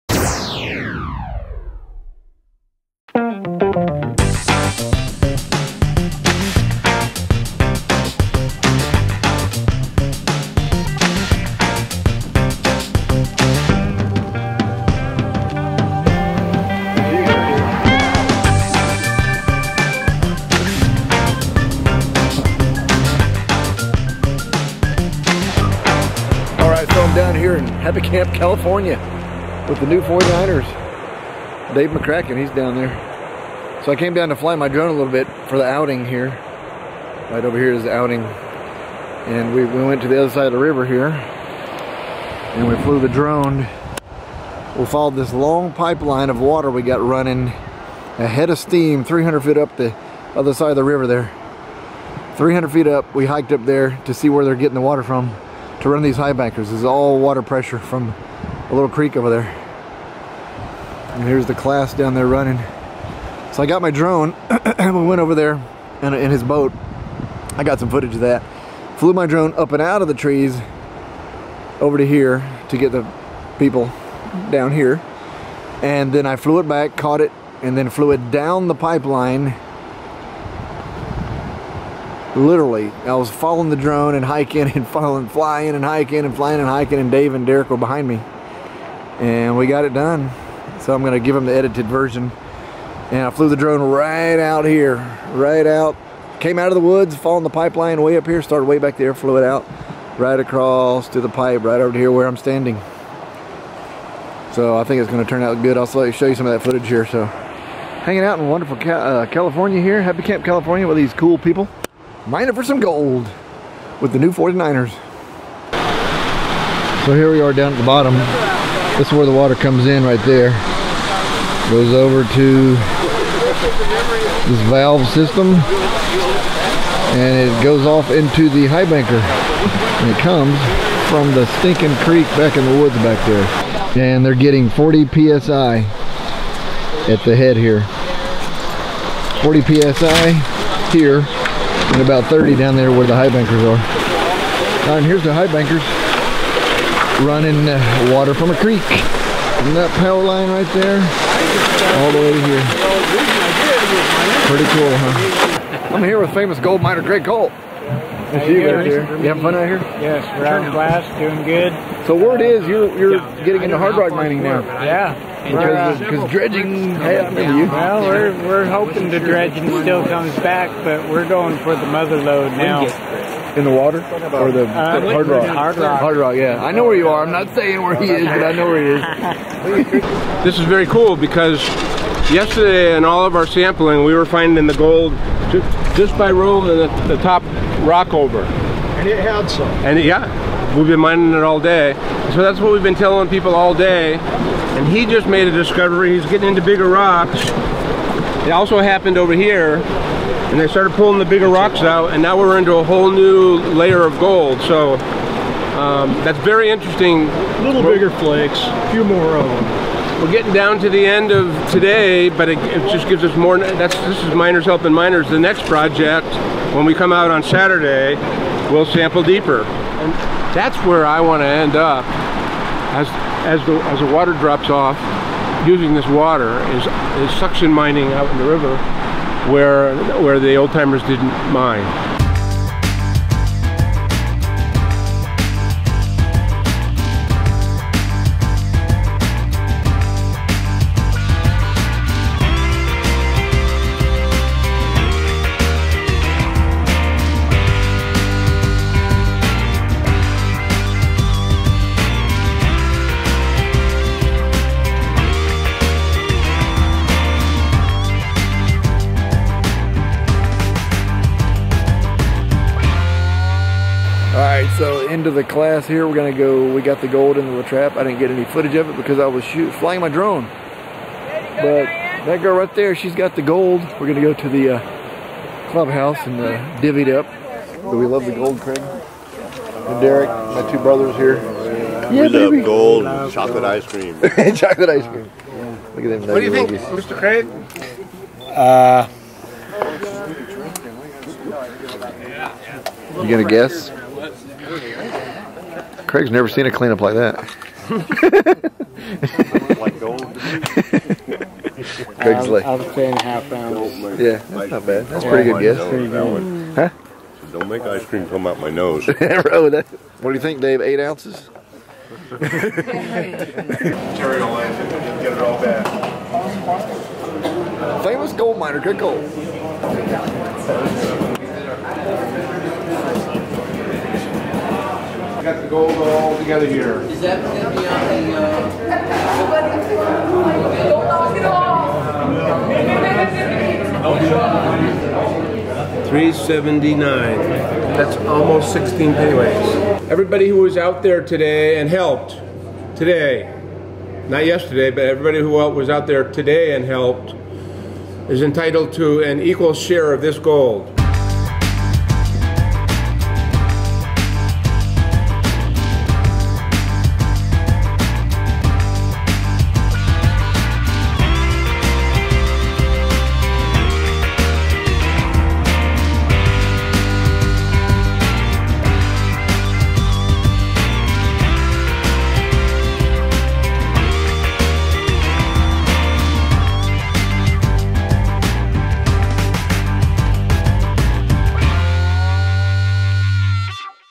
Gravity Feed Gold Mining with the New 49er's Prospecting Club | See the Gold!
We were using gravity mining method where we used water from a creek 300 fee above the river and piped it down across the river creating high pressure water to the high bankers for processing the material that was dug up. All without motors, gas or electric! Pretty cool eh?